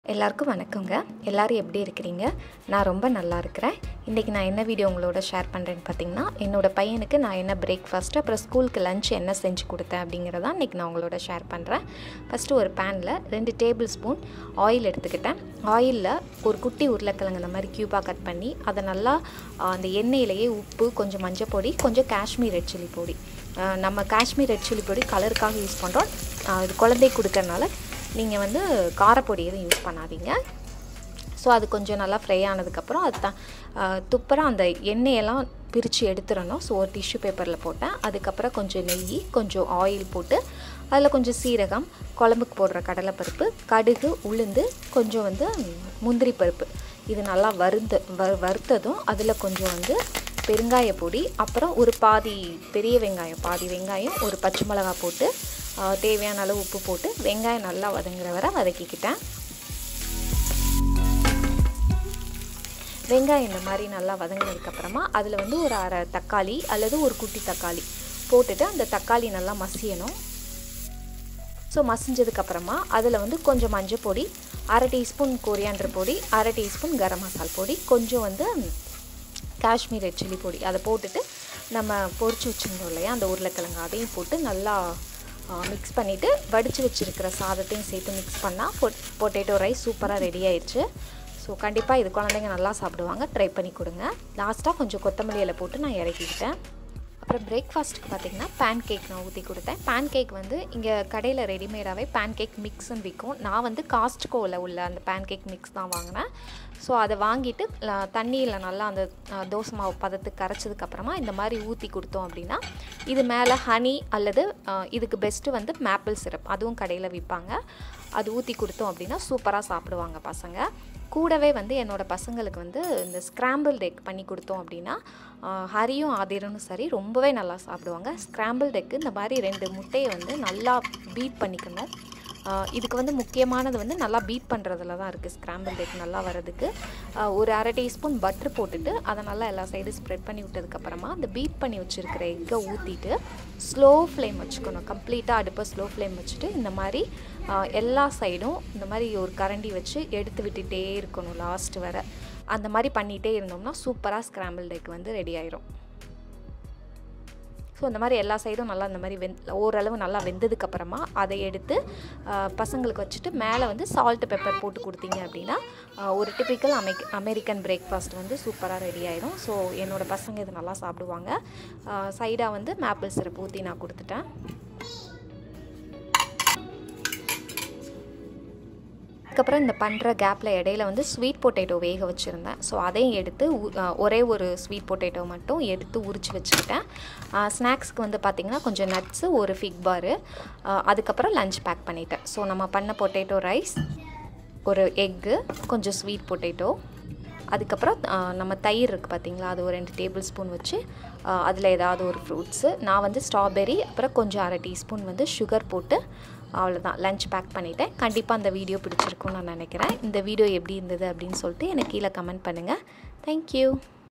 Kids Lunchbox Menu - Day 05 / Potato Rice / American Breakfast / Kala's Kitchen
Breakfast recipe of Pancake and Scrambled Egg and School lunch box as Potato Rice, Sweet Potato and Strawberry Yogurt

Starlord Kids Lunch Box Insulated Soft Lunch Bag,

Bento Boxes for Student, Thermal...